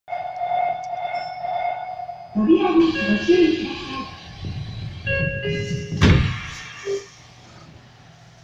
名市交N3101H(鶴舞線) N3801号車 ドア閉
6050と比べると若干柔らかめに戸締りしますね